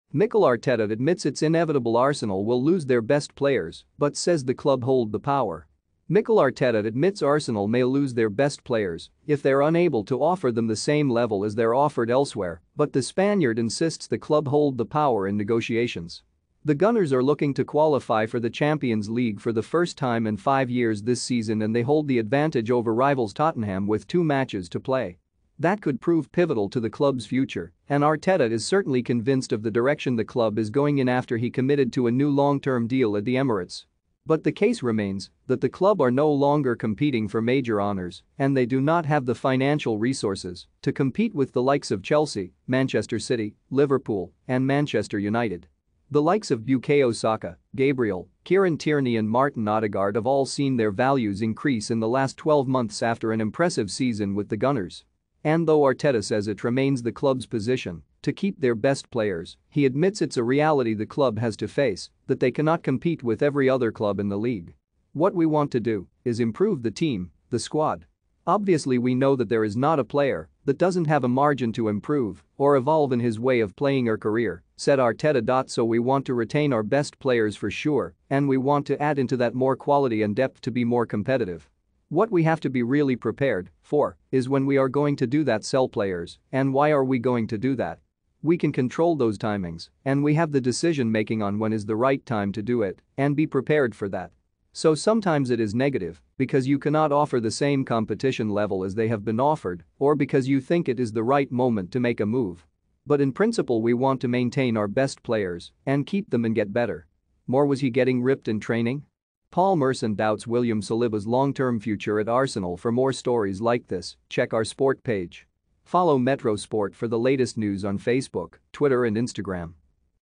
Mikel Arteta admits it’s ‘inevitable’ Arsenal will lose their best players but says the club ho
Mikel Arteta admits it’s ‘inevitable’ Arsenal will lose their best players but says the club hold the power